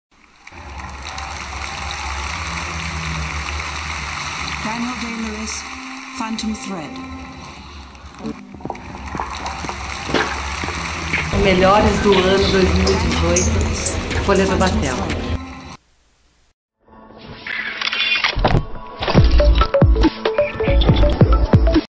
Maison Barigui - Melhores do Ano 2.018
Maison Barigui
Claudia e Clarice Dallegrave Silva  diretoras da Maison Barigui que já residiram nessa casa como seus  lares ,  resolveram transformar o local em Espaço de Eventos . Com uma área de 21 mil metros quadrados, é uma mini chácara dentro de Curitiba. Um refúgio do conceito urbano. Maison Barigui oferece infra-estrutura para eventos como lúdicas festas infantis, aniversários , casamentos e corporativos. Local ideal para sua produção inesquecível!